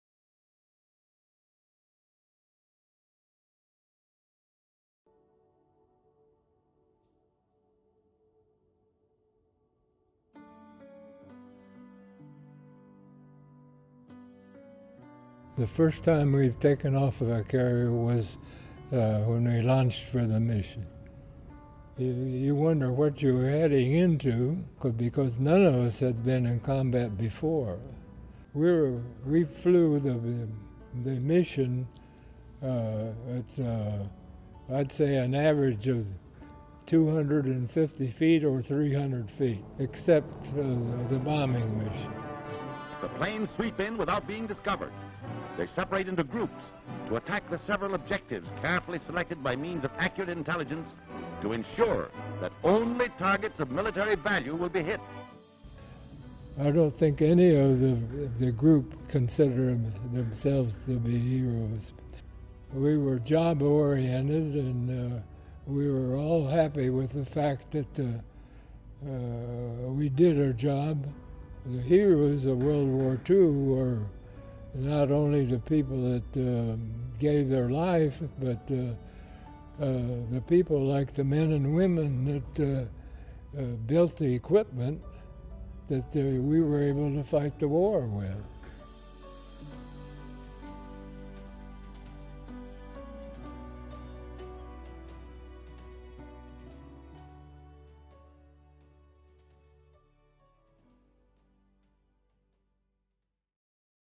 Doolittle Raid 80th Anniversary
The Doolittle Raid, also known as the Tokyo Raid, was an air raid on 18 April 1942 by the United States on the Japanese capital Tokyo and other places on Honshu during World War II.

DC, UNITED STATES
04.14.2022

It was the first air operation to strike the Japanese archipelago, and served as retaliation for the 7 December 1941 attack on Pearl Harbor. The raid was planned by, led by, and named after Lieutenant Colonel James Doolittle. Narration by Col Dick Cole.

Film Credits: Video by Juan Femath
Air Force Television Pentagon (SAF/PAI)